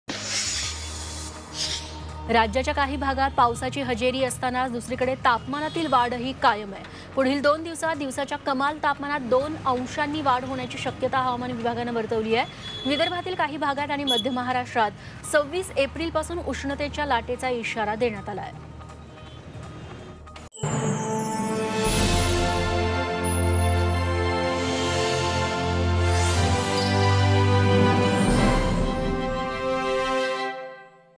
काही भागांमध्ये पावसाची हजेरी, मात्र तापमानातील वाढ कायम; कमाल तापमानात 2 अंशांच्या वाढीची शक्यता
राज्याच्या काही भागांमध्ये पावसाची हजेरी असतानाच दुसरीकडे तापमानातील वाढ कायम आहे... पुढील दोन दिवसांत दिवसाच्या कमाल तापमानात 2 अंशांनी वाढ होण्याची शक्यता हवामान विभागानं वर्तवलीये... विदर्भातील काही भागात आणि मध्य महाराष्ट्रात 26 एप्रिलपासून उष्णतेच्या लाटेची स्थिती राहणाराय...

5 Minitat 25 Batmya । 5 मिनिटांत 25 बातम्या